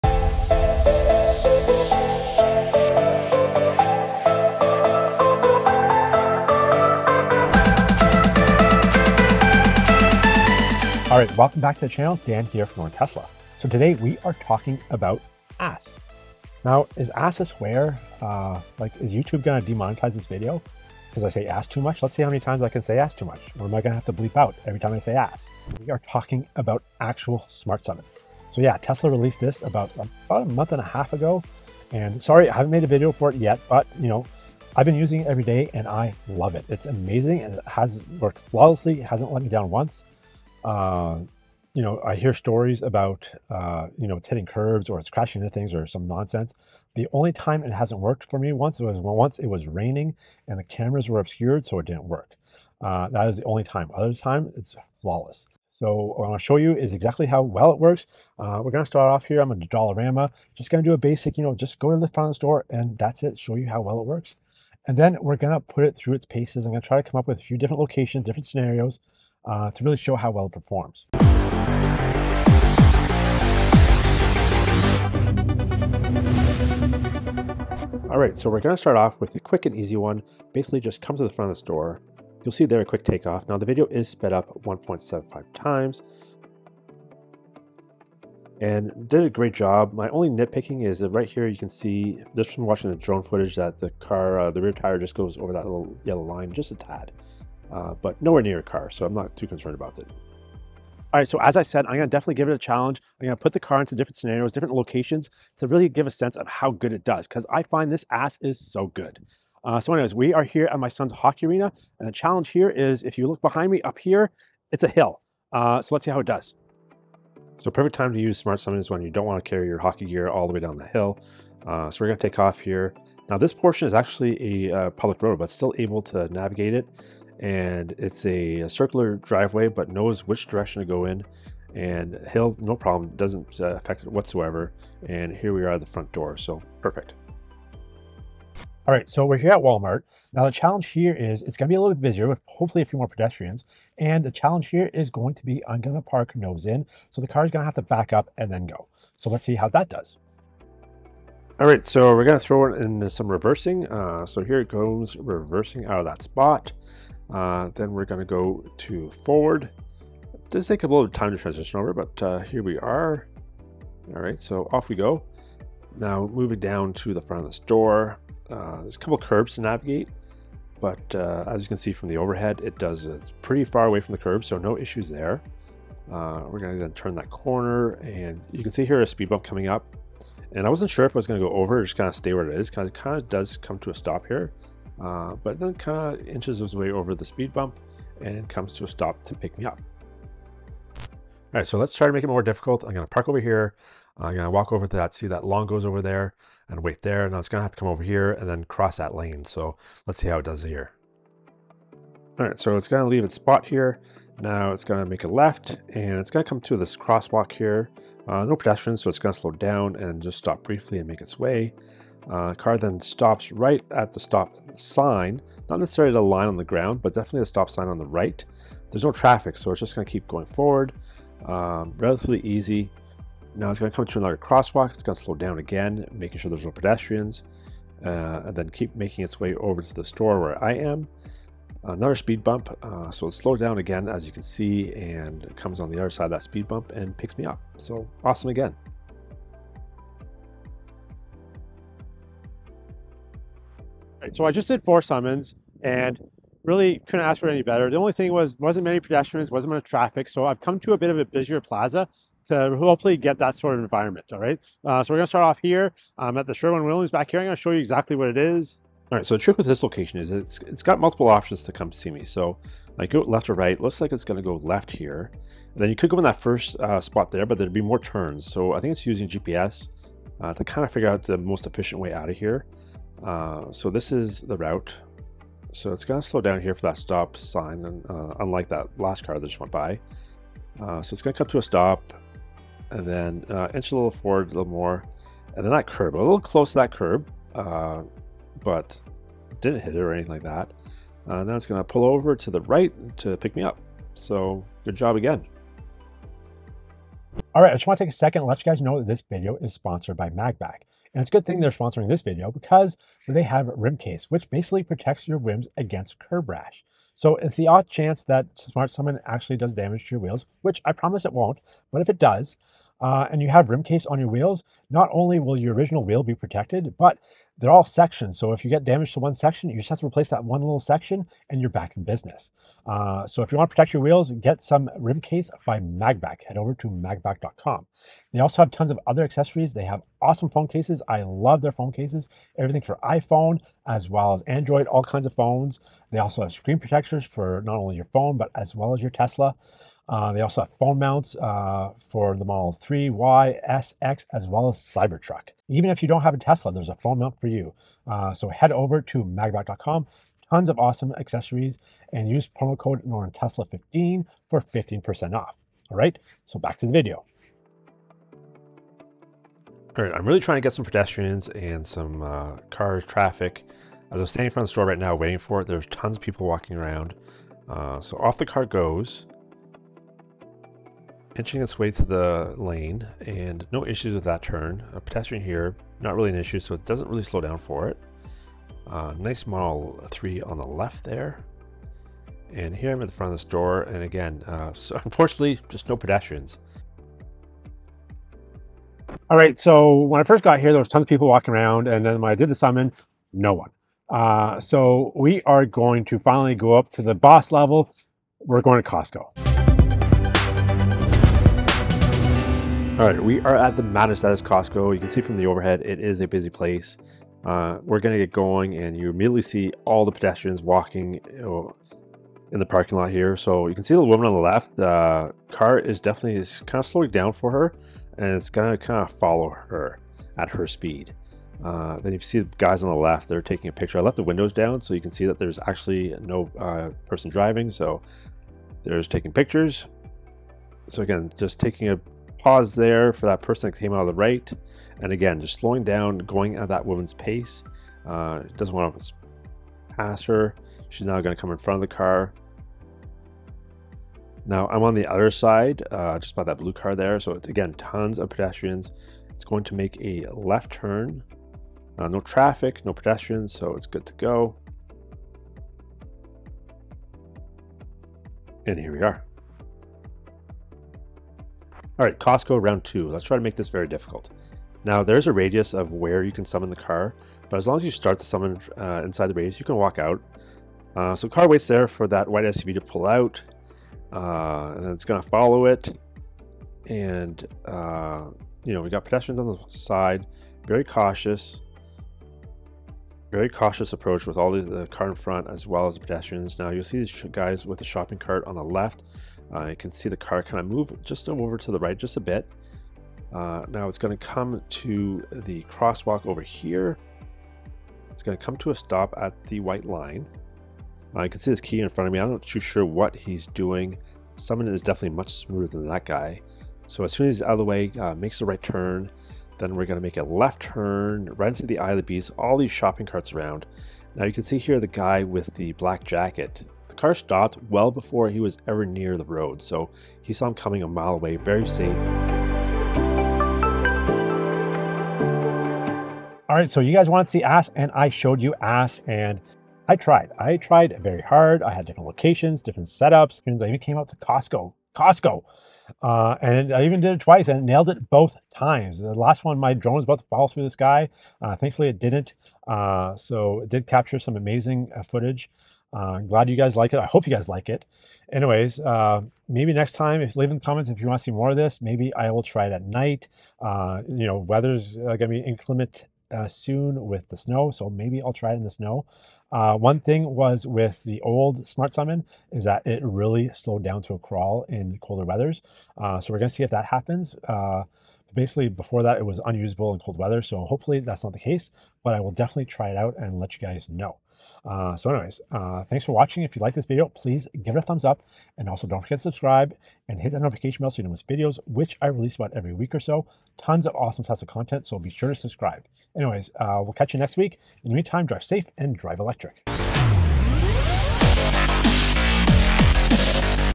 Tesla ASS (Actually Smart Summon) Showdown: From Simple to Insane!
Watch Tesla's Actually Smart Summon in action! From beginner to expert level, see how well Tesla's latest Summon feature navigates parking lots. Witness the evolution from simple to complex scenarios, showcasing the true capabilities of Tesla's autonomous tech.